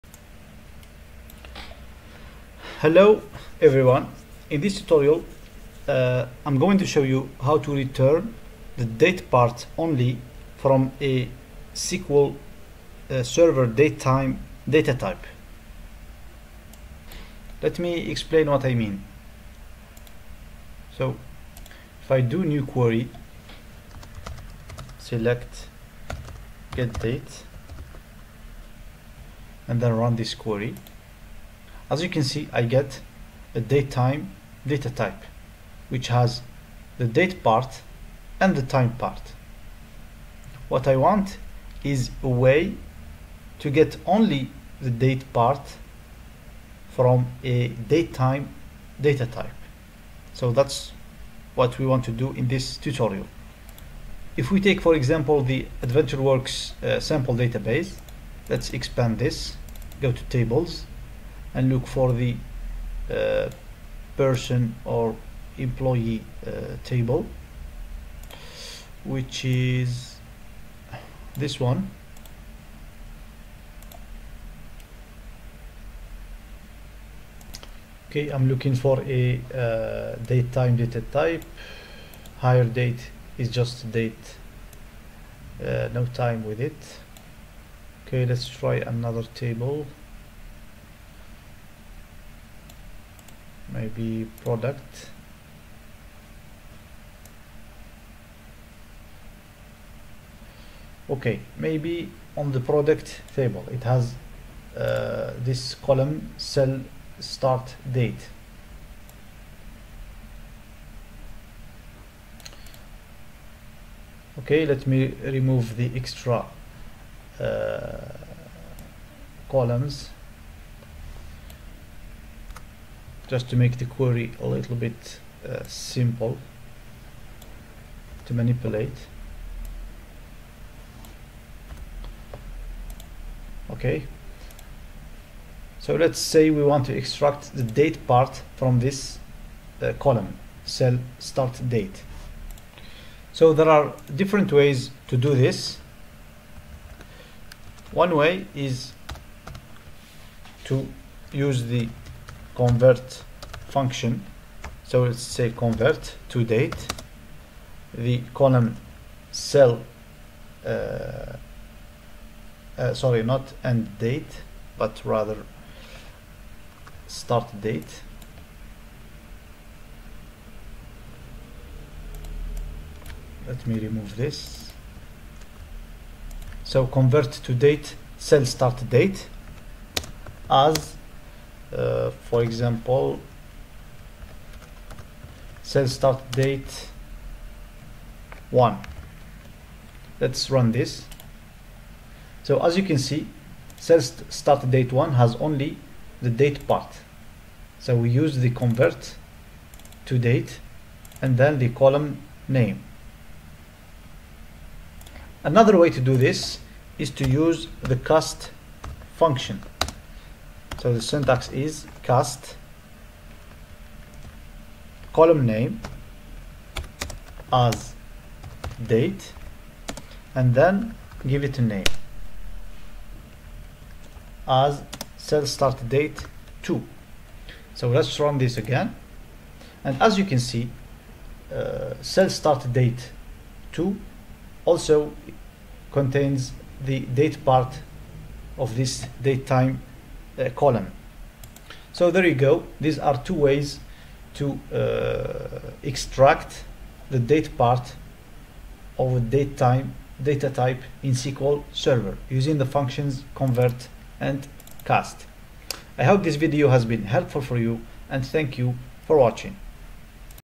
How to Return Date Part Only from a SQL Server Datetime datatype
in this tutorial, we will see how to extract the date part only of a datetime column in SQL Server using 2 methods